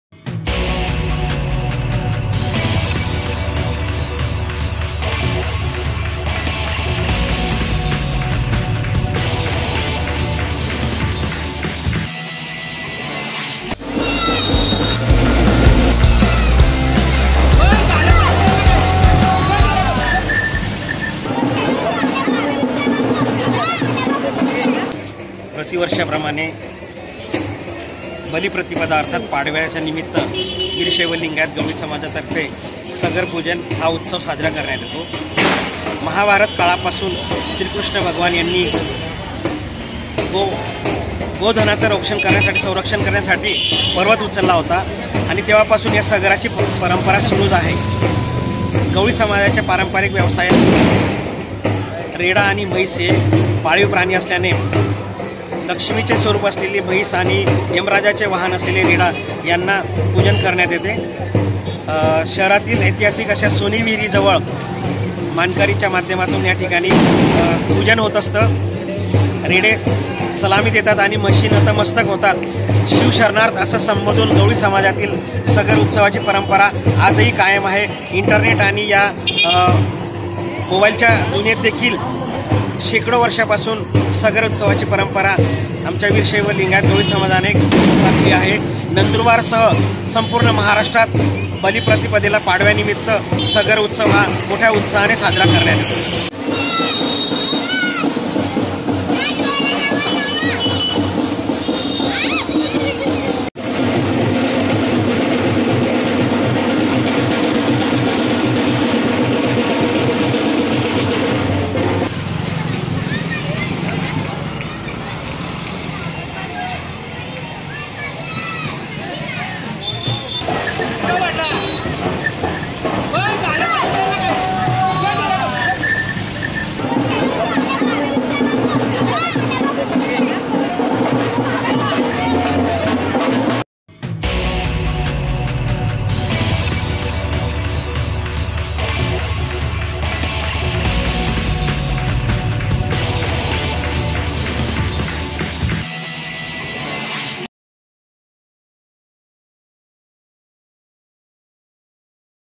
नंदुरबार, गवळी समाजाच्या वतीने म्हैस आणि रेडा यांची पूजा करून 'सदर' उत्सव साजरा करण्यात आला.
बलिप्रतिपदा अर्थात पाडव्याच्या निमित्ताने दरवर्षी गवळी समाजाच्या वतीने नंदुरबार शहरात 'सदर' उत्सव मोठ्या उत्साहाने साजरा केला जातो. म्हैस आणि रेडा यांचे पूजन करून साजरा केला जाणारा या उत्सवाला नंदनगरीत विशेष आकर्षण असते. कोरोनाच्या पार्श्वभूमीवर यंदाही गवळी समाजाच्या वतीने 'सदर' उत्सव साध्या पद्धतीने साजरा करण्यात आला. महाभारत काळापासून भगवान श्रीकृष्ण यांनी गोधनाची संरक्षण करण्यासाठी पर्वत उचलला होता; तेव्हापासून या 'सदर' उत्सवाची परंपरा सुरु झाली होती, ती आजपर्यंत चालू आहे. गवळी समाजाच्या पारंपरिक व्यवसायामध्ये म्हैस आणि रेडा हे पाळीव प्राणी आहेत. लक्ष्मीचे स्वरूप असलेली म्हैस आणि यमराजाचे वहन रेडा यांची शहरातील ऐतिहासिक सोनी विहिरीजवळ मानकरींच्या उपस्थितीत विधिवत पूजा केली जाते. दरम्यान रेडे सलामी देतात तर म्हैस नतमस्तक होतात. मोबाईल इंटरनेटच्या युगातही गवळी समाजाच्या वतीने बलिप्रतिपदा पाडवा निमित्त सदर उत्सव साजरा करण्याची परंपरा कायम ठेवली आहे.